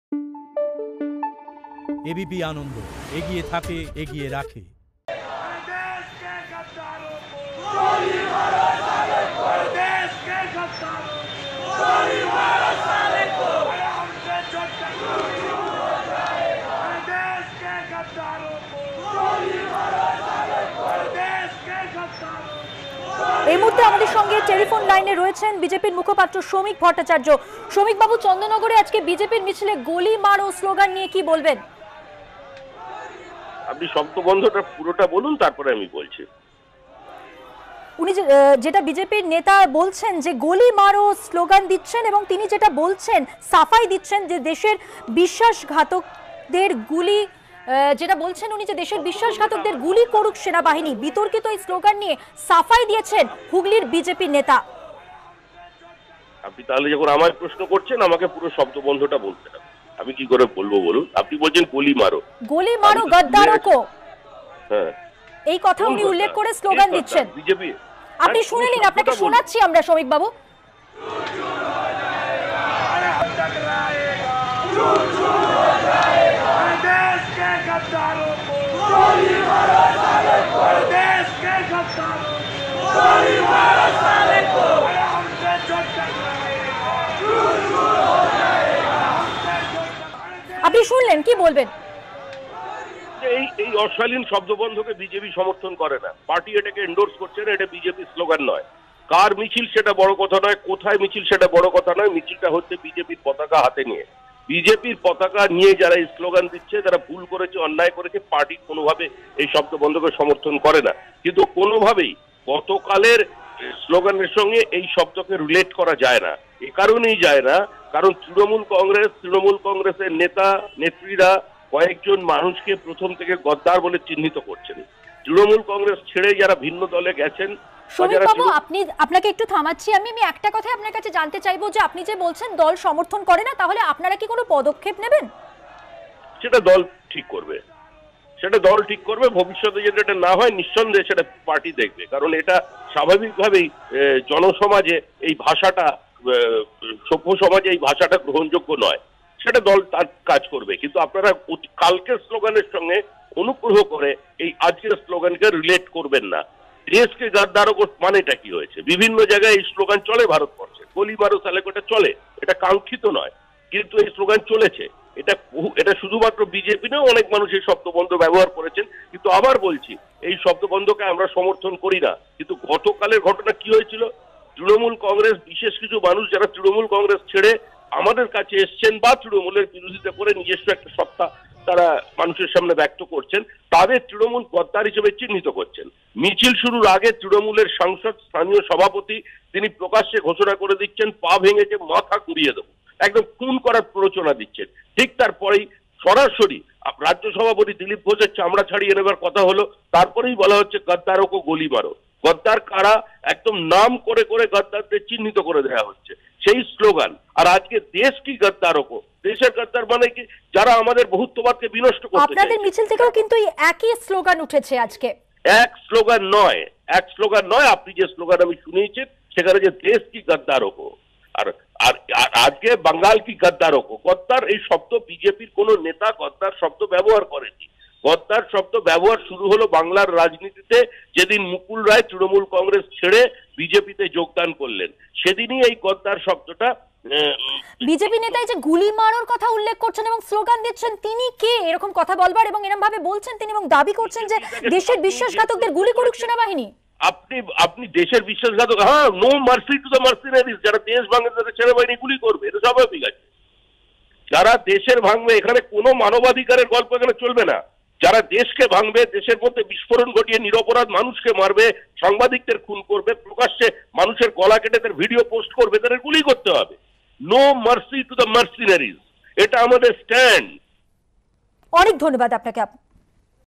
TMC-র পর এবার Suvendu Adhikari-র র‍্যালিতেও ‘গোলি মারো’ স্লোগান
তৃণমূলের পর এবার শুভেন্দু অধিকারীর র‍্যালিতেও ‘গোলি মারো’ স্লোগান! চন্দননগরে বিজেপির মিছিলেও ‘গোলি মারো’ স্লোগান। চন্দননগরের রথতলা মোড়ের কাছে সেই বিতর্কিত স্লোগান। ‘দেশের বিশ্বাসঘাতকদের গুলি করুক সেনা বাহিনী।’ বিতর্কিত স্লোগান নিয়ে সাফাই হুগলির বিজেপির নেতার।
গদ্দারদের গুলি মারাই উচিত। এটাই আমাদের নৈতিক অবস্থান। বিজেপি নেতা সুরেশ সাউয়ের পাশে দাঁড়িয়ে মন্তব্য শমীক ভট্টাচার্যর।
প্রকাশ্যে মিছিলে গোলি মারো স্লোগান সমর্থনযোগ্য নয়। মন্তব্য করলেন তৃণমূলের মুখপাত্র কুণাল ঘোষ।